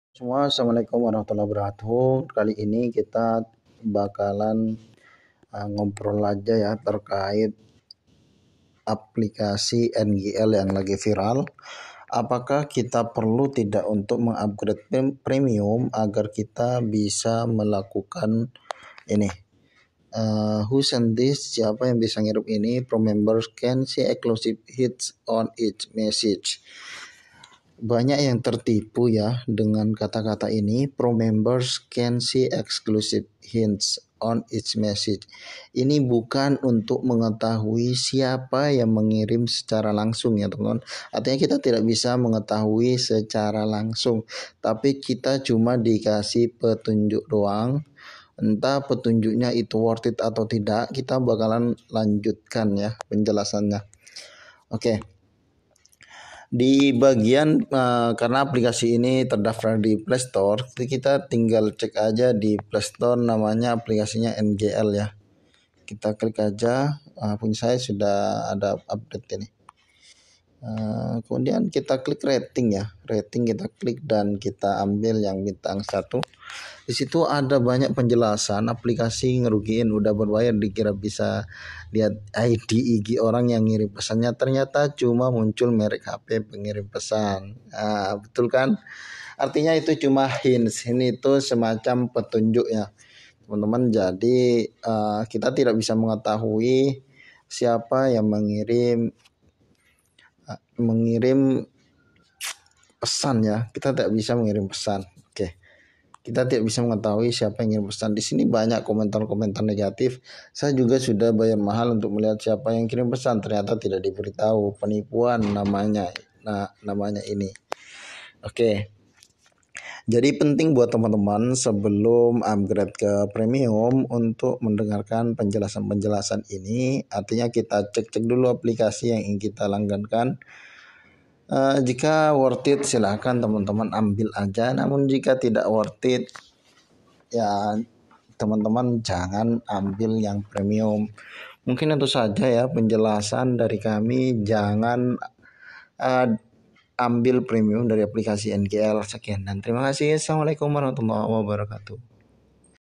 Penting Jangan Update Pro Aplikasi NGL Mengetahui Pengirim Balasan NGL Anonymous Message
Informasi terkait update pro dari aplikasi mgl apakah memang benar bisa mengetahui siapa pengirim balasan ngl Andromax message.

Bagi Anda yang ingin mencoba untuk update please tonton video ini sebelum melakukan langganan terhadap aplikasi ngl pro.